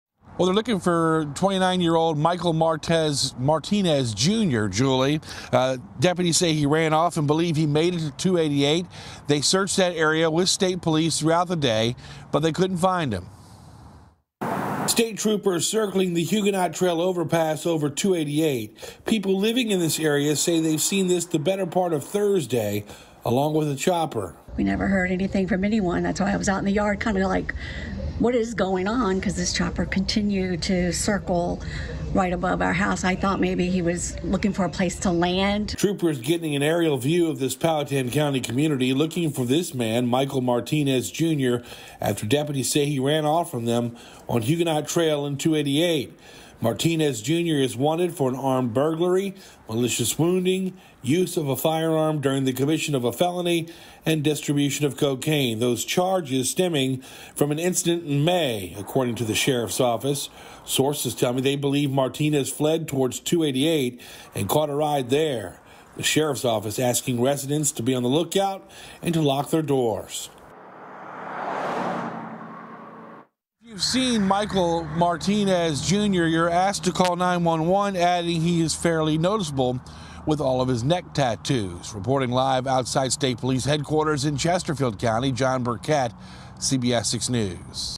Powhatan County Sheriff searching for man after he escaped police custody
The Powhatan County Sheriff's Office is searching for a wanted man after he fled police when they tried to take him into custody Thursday morning.